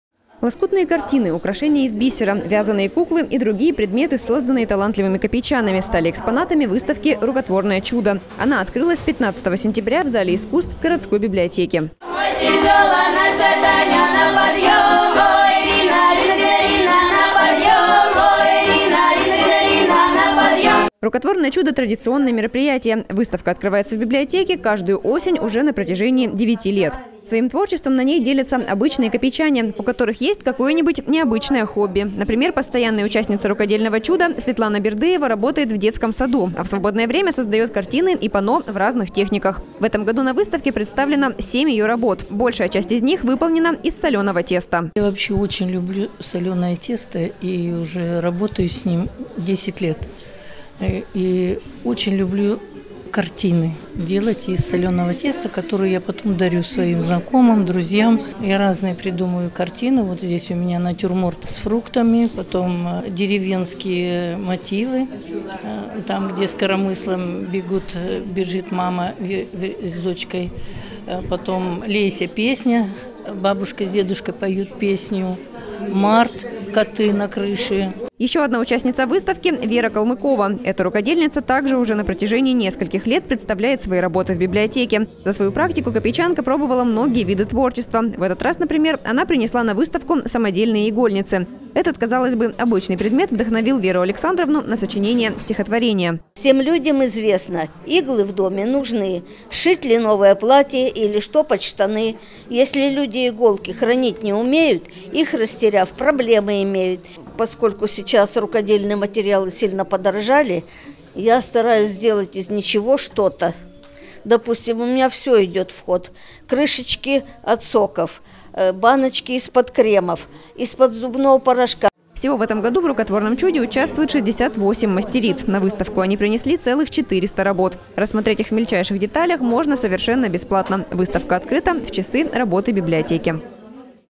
В библиотеке открылась выставка «Рукотворное чудо»
На выходных в городской библиотеке открылась выставка «Рукотворное чудо». Ее экспонатами стали 400 работ, которые были созданы руками 68 талантливых копейчан.